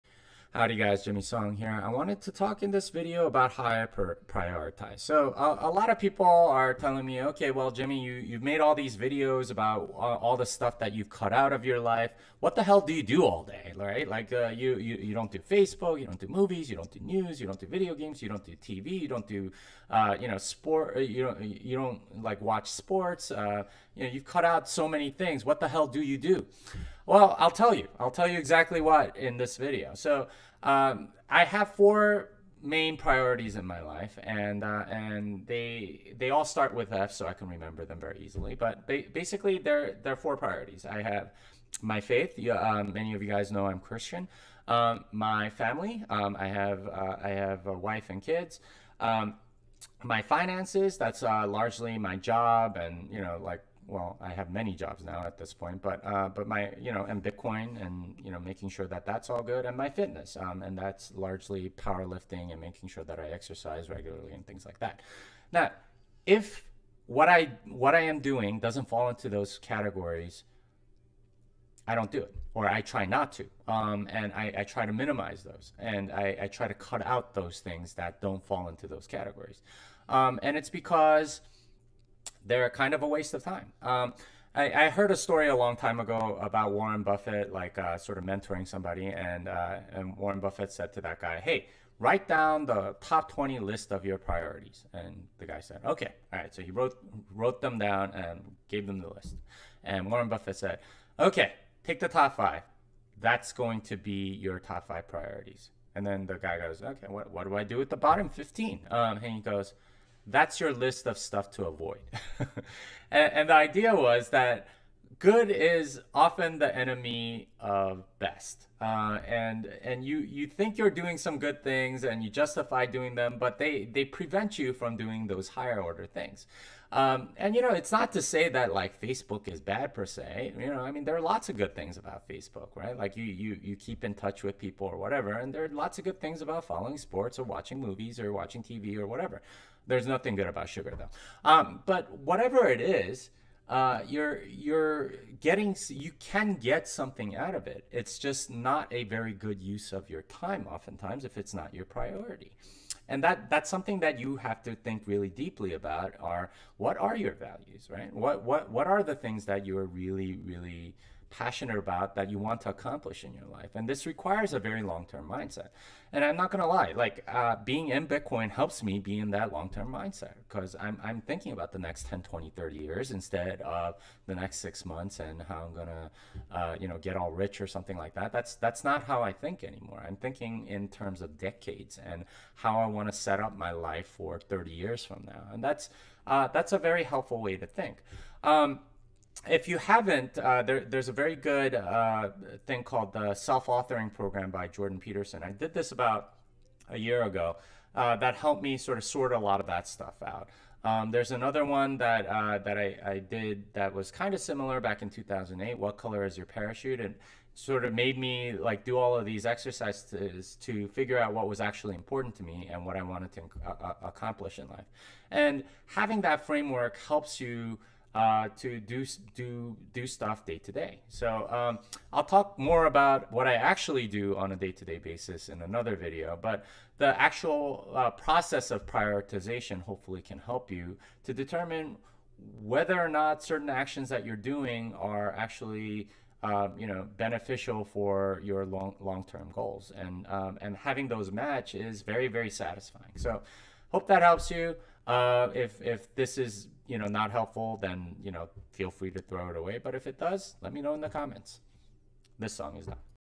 My four F's in life a.k.a. priorities!
I talk about how I prioritize actions to determine how they fit my long-term goals.

Programming Bitcoin
The Little Bitcoin Book
Thank God for Bitcoin